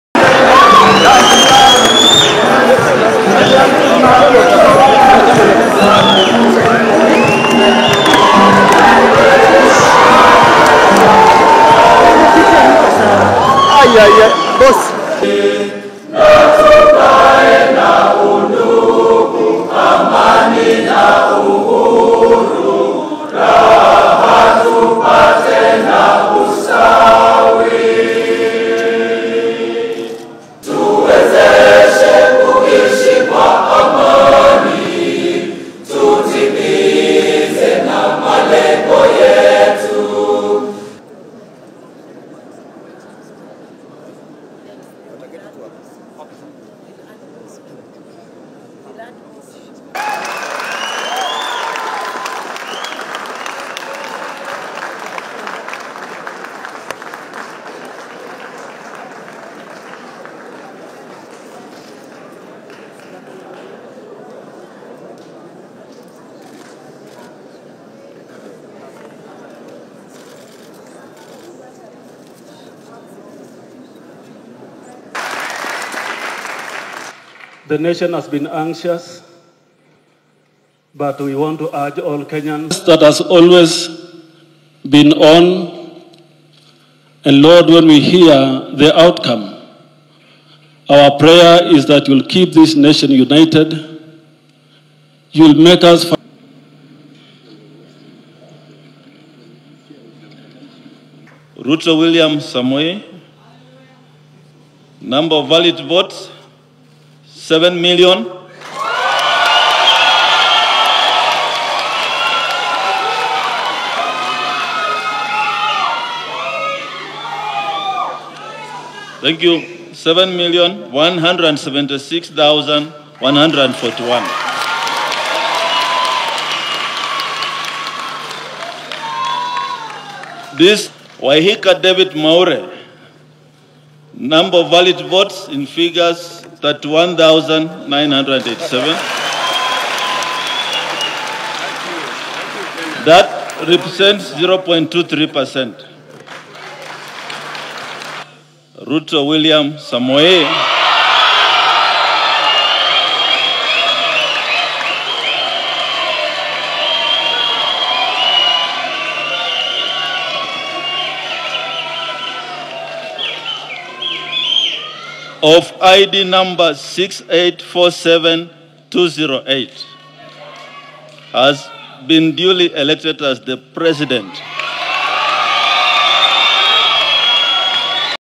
The Deputy Inspector General of Police Noor Gabow taking Charge at the Bomas of Kenya#livestream
The Unsung hero. Noor Gabow